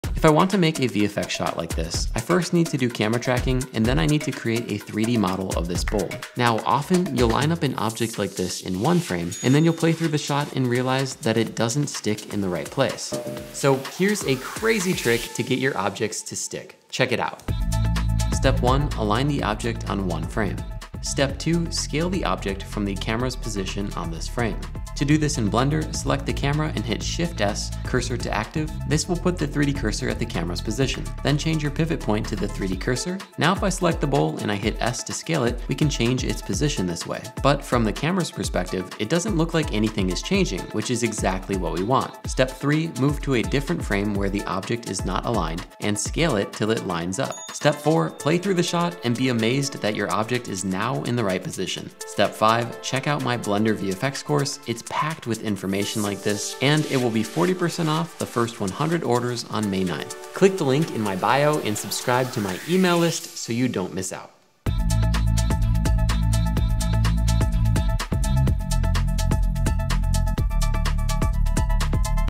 Stop objects from sliding in your VFX shots with this simple placement trick✨
Ever struggle to get your 3D elements in the right position so they stick properly in your VFX shots? Check out this trick that uses the camera's position as a pivot point for easy and accurate object placement. It works like magic! 🪄